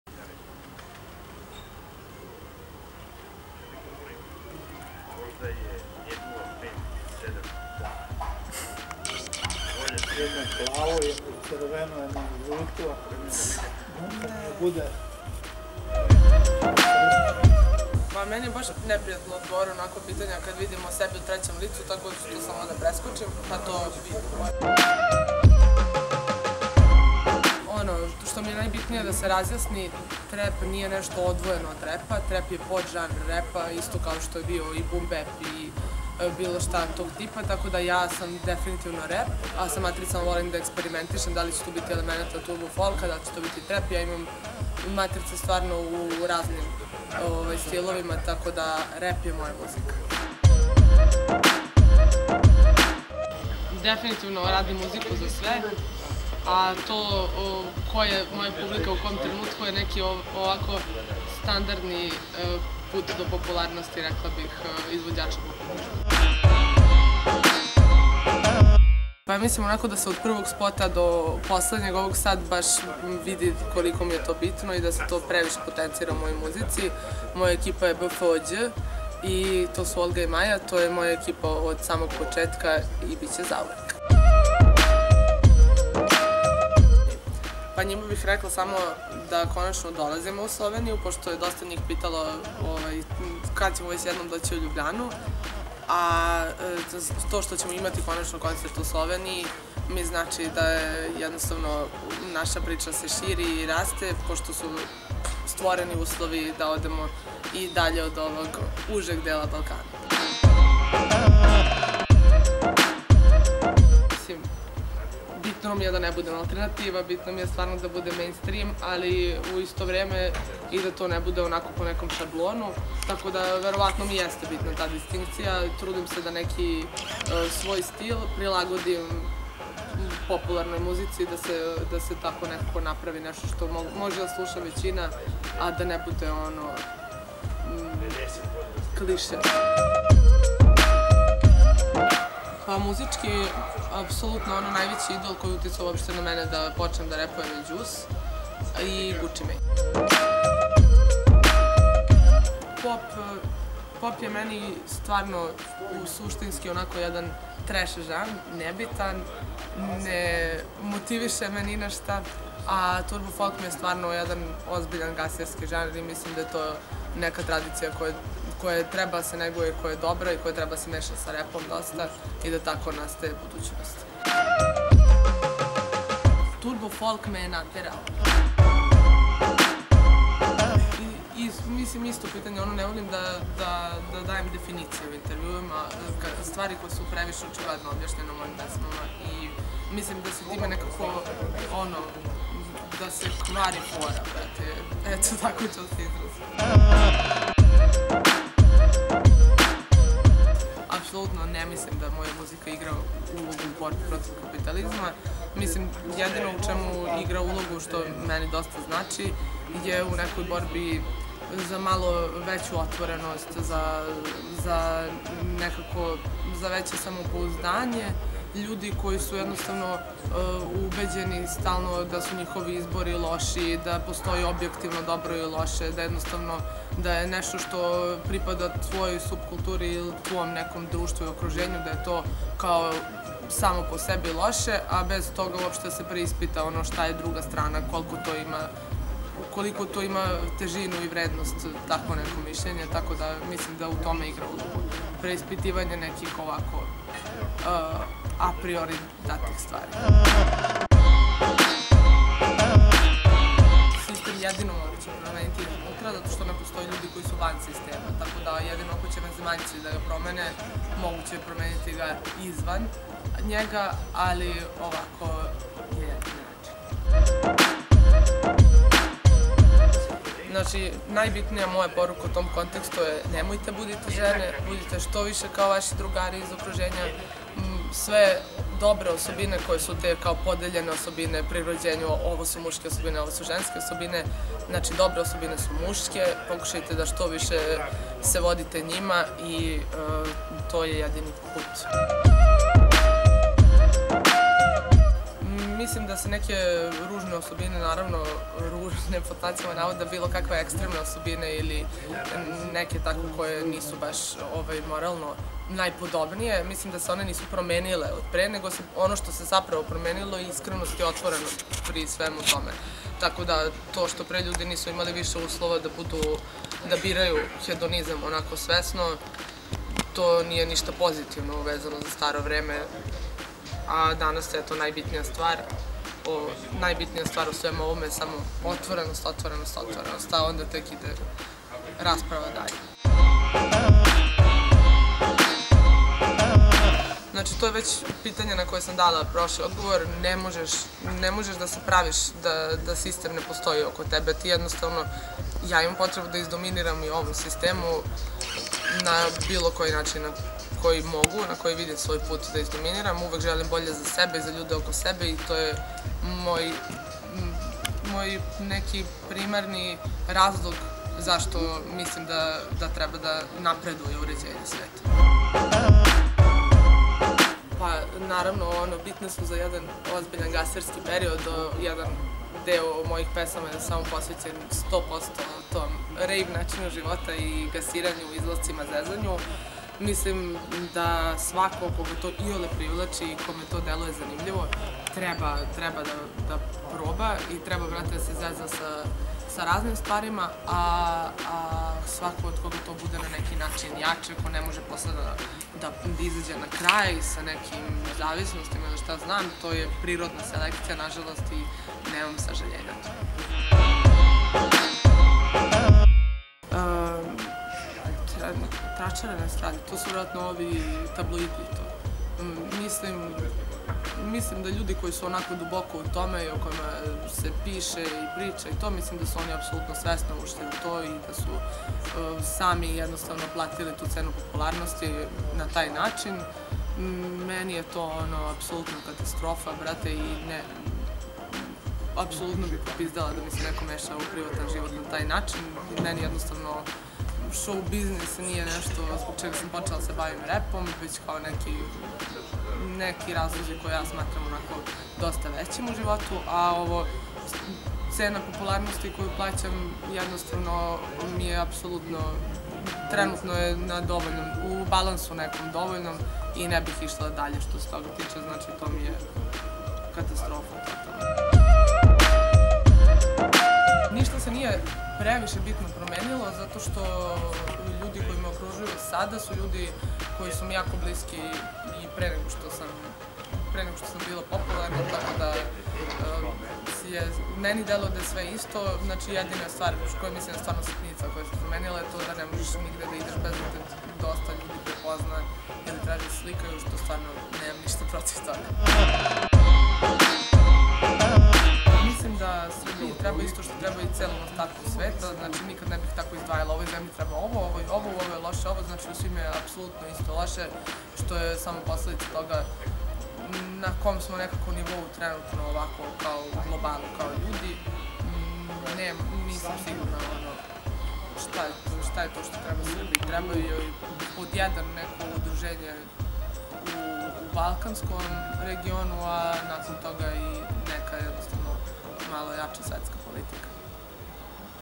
Mimi Mercedez - Intervju za Radio Študent
Promo intervju povodom prvog nastupa u Sloveniji - 19.5. Studentski kampus, Ljubjana.